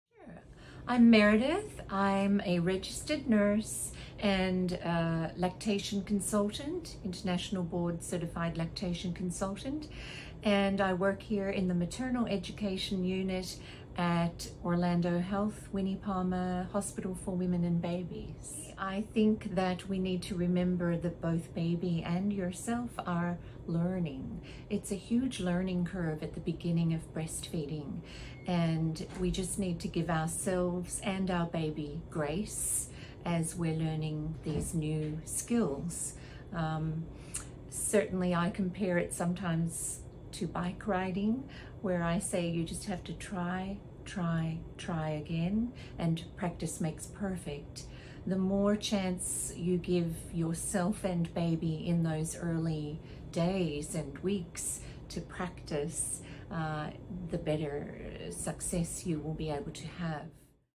Breastfeeding
Meredith Harewood, a registered nurse and board-certified lactation consultant, shares how the learning curve for mother and baby requires grace and plenty of practice.

Learn more about breastfeeding at the Orlando Health Winnie Palmer Hospital for Women and Babies Postpartum Series, where we cover topics to support your well-being and parenthood.